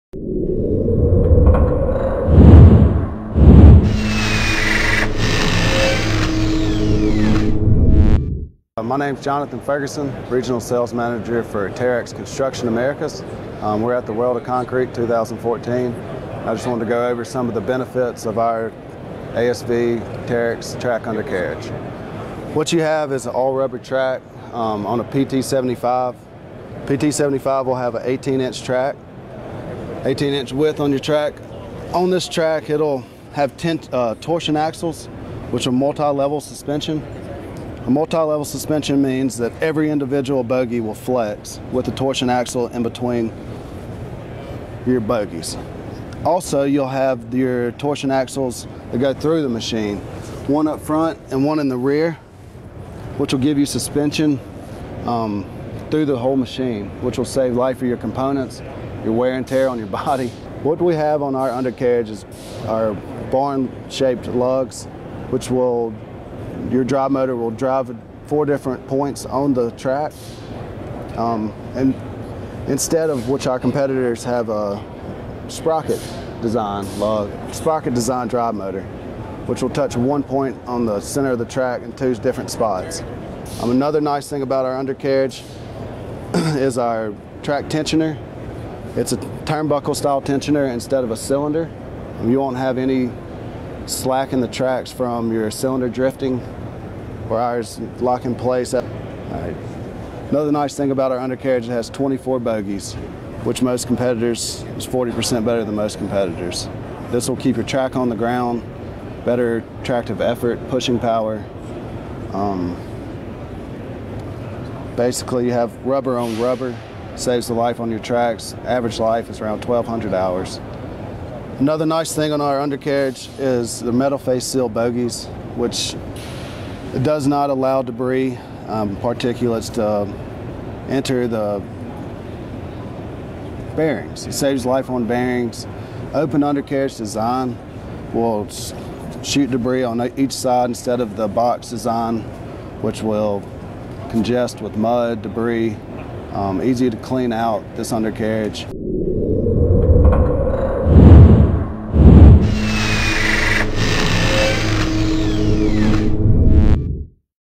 What Sets the Terex ASV Undercarriage Apart
Terex talks about the unique features that lift the undercarriage life of its compact track loaders to average 1,200 hours